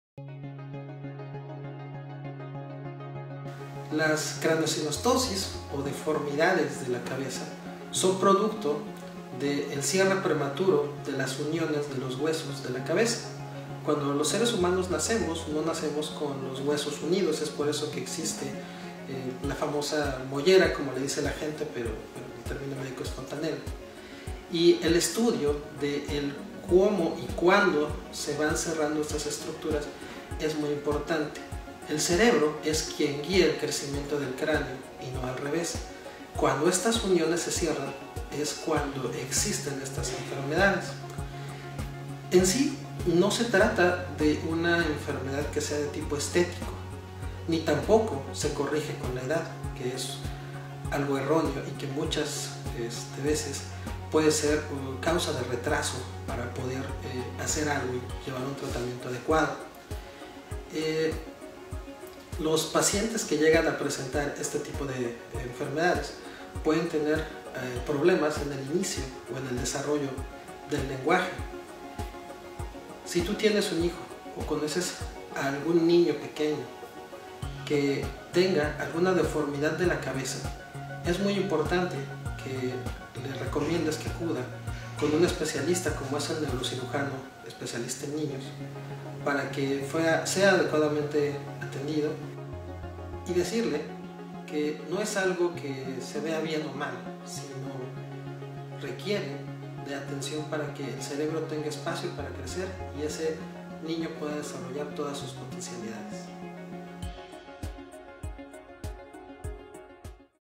Craneosinostosis - Todo lo que necesitas saber con el Dr. Alberto Hernández Hernández
BlueNetHospitals Los Cabos

La Craneosinostosis o deformidades de la cabeza, es un defecto congénito en el que se produce un cierre prematuro de las uniones de los huesos de la cabeza.

Existen articulaciones de tejido fibroso que conectan los huesos del cráneo, a este tejido fibroso se le denomina “suturas”. Las suturas, permiten que el tamaño del cráneo se adapte a medida de que el cerebro crece en su interior. El cerebro continúa poco a poco desarrollándose durante los primeros años de vida del bebé. Conforme la persona va creciendo, estas suturas que permanecían flexibles ahora se endurecen y se cierran. Cuando las suturas se cierran, el cráneo ya crece más.

Cuando estas suturas se cierran demasiado pronto, se produce la Craneosinostosis, restringiendo la capacidad del cráneo para desarrollarse y crecer de forma normal; como a pesar de esto el cerebro no deja de crecer, se provoca una apariencia deforme en la cabeza del bebé.